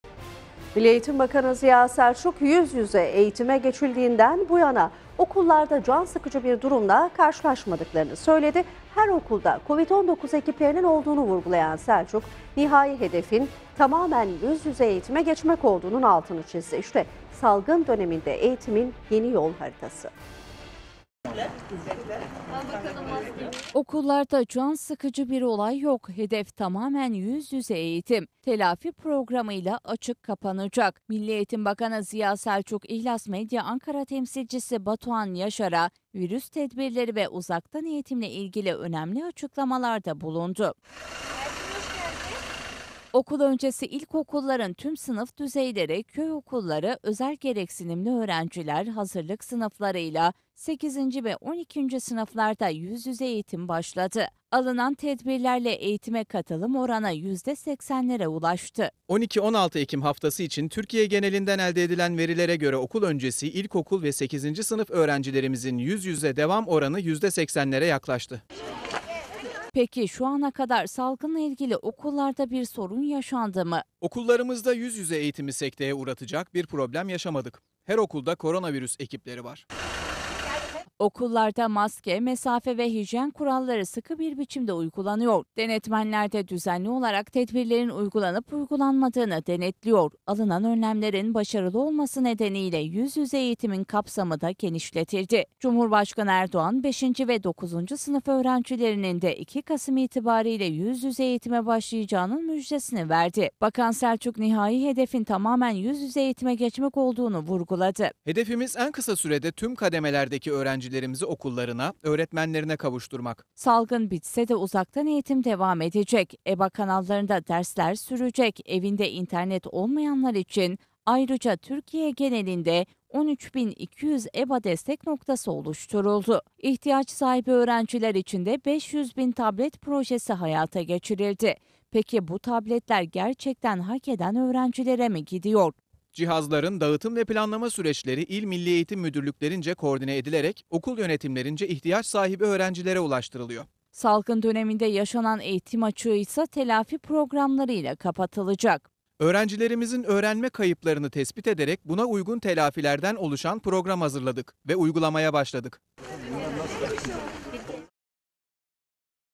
Salgın Döneminde Eğitimin Yeni Yol Haritası
Milli Eğitim Bakanı Ziya Selçuk, yüz yüze eğitime geçildiğinden bu yana okullarda can sıkıcı bir durumla karşılaşmadıklarını söyledi. Her okulda Kovid-19 ekiplerinin olduğunu vurgulayan Selçuk, nihai hedefin tamamen yüz yüze eğitime geçmek olduğunun altını çizdi. İşte salgın döneminde eğitimin yeni yol haritası…